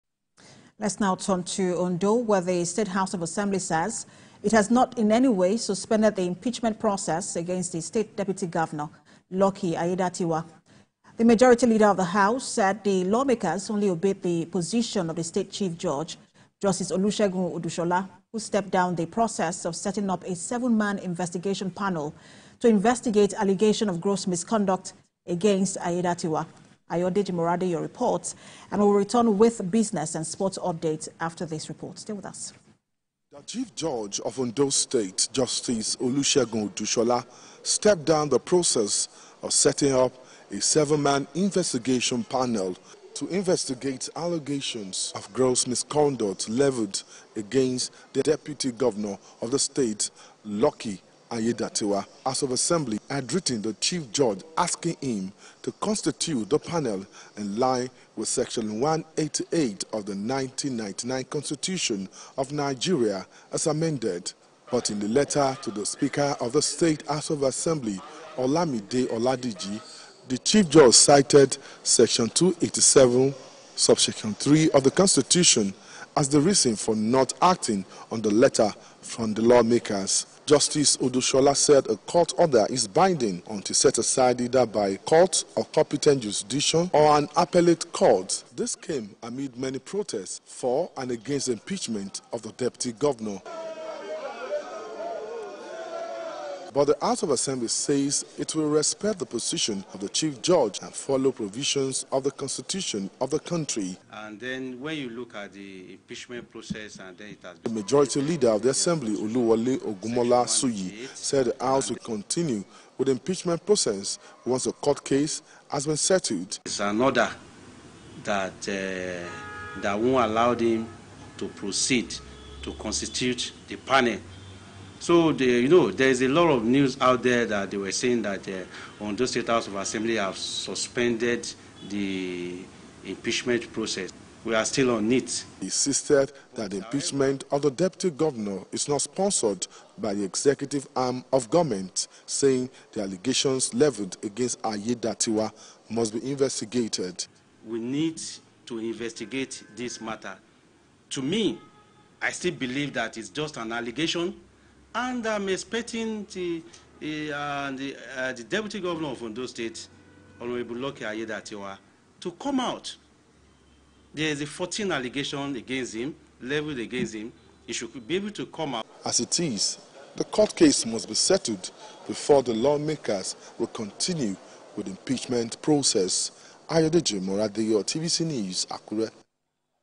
Ondo Assembly Says It Has No Plan To Suspend Process To Impeach Deputy Governor, Lucky Aiyedatiwa
The Ondo State House of Assembly says it has not in any way suspended the impeachment process against the State Deputy Governor, Lucky Aiyedatiwa.

The Majority Leader of the House, Oluwole Ogunmolasuyi said the lawmakers only obeyed the position of the State Chief Judge, Justice Olusegun Odusola, who stepped down the process of setting up a seven-man investigation panel to investigate allegation of gross misconduct against Aiyedatiwa.